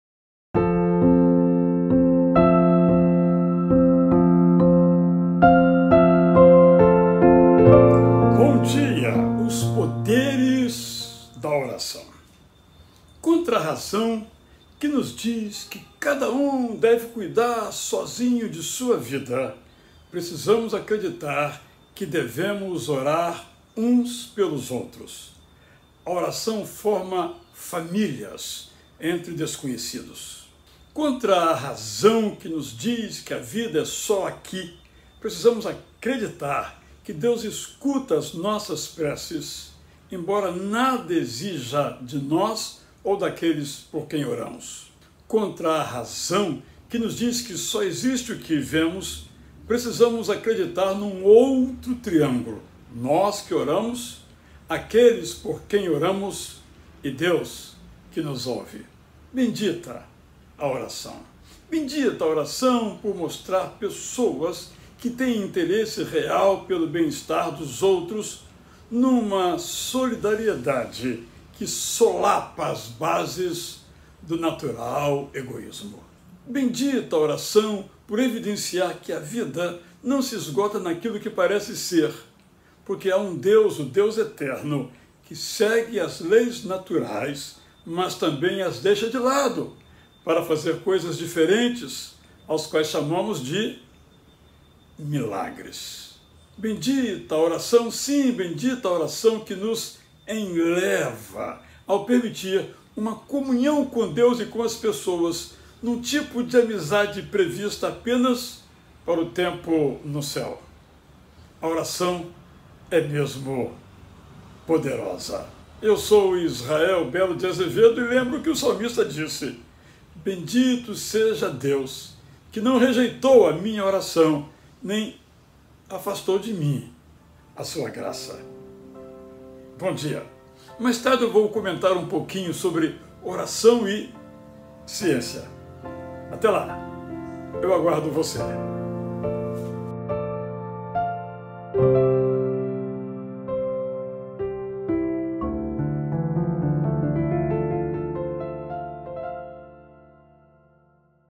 (BOM DIA AMIGO, 3196) Os poderes da oração
“Vá onde suas melhores orações o levam”. (Frederick Buechner, 1926-)

Os poderes da oração

Contra a razão, que nos diz que cada um deve cuidar sozinho de sua vida, precisamos acreditar que devemos orar uns pelos outros. A oração forma famílias entre desconhecidos.
Contra a razão, que nos diz que a vida é só aqui, precisamos acreditar que Deus escuta as nossas preces, embora nada exija de nós ou daqueles por quem oramos.
Contra a razão, que nos diz que só existe o que vemos, precisamos acreditar num outro triângulo: nós, que oramos; aqueles por quem oramos, e Deus, que nos ouve.
Bendita a oração, por mostrar pessoas que têm interesse real pelo bem-estar dos outros, numa solidariedade que solapa as bases do natural egoísmo.
Bendita a oração, por evidenciar que a vida não se esgota naquilo que parece ser, porque há um Deus, o Deus Eterno, que segue as leis naturais, mas, também, as deixa de lado para fazer coisas diferentes, aos quais chamamos de milagres.
Bendita a oração, que nos faz subir, ao permitir uma comunhão com Deus e com as pessoas,  num tipo de amizade previsto apenas para o tempo no céu.
A oração é mesmo poderosa.

“Bendito seja Deus, que não rejeitou a minha oração, nem afastou de mim a sua graça”. (Salmo 66.20)

Bom dia!